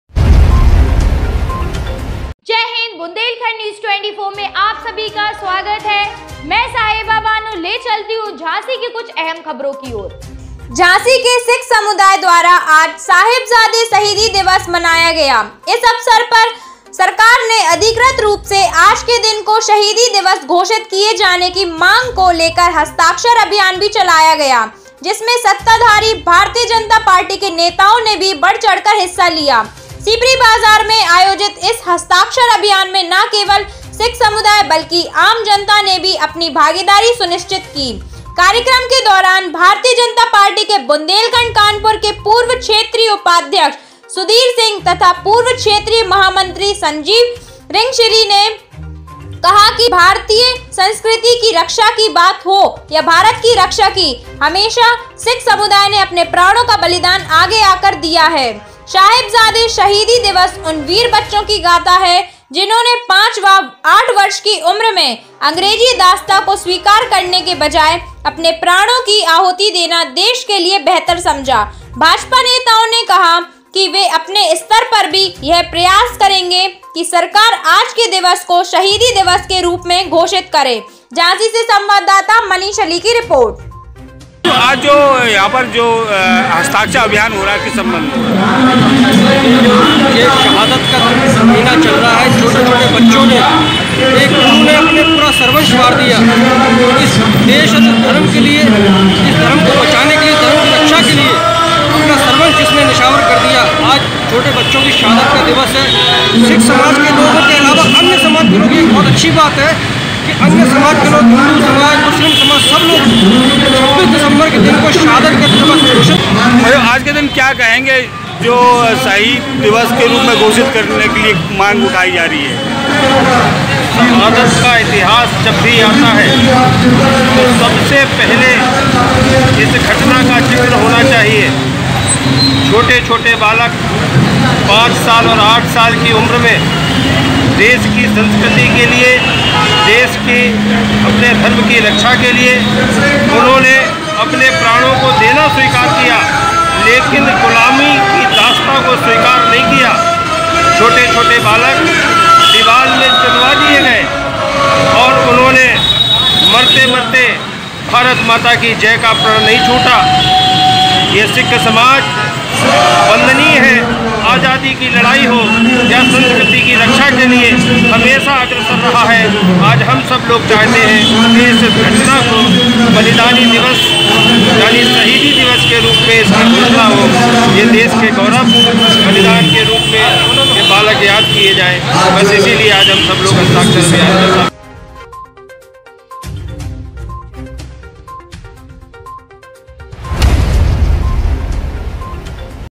झांसी में सिख समुदाय द्वारा आज साहिबजादे शहीदी दिवस मनाया गया
झांसी के सिख समुदाय द्वारा आज साहिबजादे शहीदी दिवस मनाया गया। इस अवसर पर सरकार से अधिकृत रूप से आज के दिन को शहीदी दिवस घोषित किए जाने की मांग को लेकर हस्ताक्षर अभियान भी चलाया गया, जिसमें सत्ताधारी भारतीय जनता पार्टी के नेताओं ने भी बढ़-चढ़कर हिस्सा लिया। सीपरी बाजार में आयोजित इस हस्ताक्षर अभियान में न केवल सिख समुदाय, बल्कि आम जनता ने भी अपनी भागीदारी सुनिश्चित की। कार्यक्रम के दौरान भारतीय जनता पार्टी के बुंदेलखंड-कानपुर के पूर्व क्षेत्रीय उपाध्यक्ष सुधीर सिंह तथा पूर्व क्षेत्रीय महामंत्री संजीव श्रृंगीऋषि ने कहा कि भारतीय संस्कृति की रक्षा की बात हो या भारत की रक्षा की, हमेशा सिख समुदाय ने अपने प्राणों का बलिदान आगे आकर दिया है। साहिबजादे शहीदी दिवस उन वीर बच्चों की गाथा है, जिन्होंने 5 व 8 वर्ष की उम्र में अंग्रेजी दास्तां को स्वीकार करने के बजाय अपने प्राणों की आहुति देना देश के लिए बेहतर समझा। भाजपा नेताओं ने कहा कि वे अपने स्तर पर भी यह प्रयास करेंगे कि सरकार आज के दिवस को शहीदी दिवस के रूप में घोषित करे।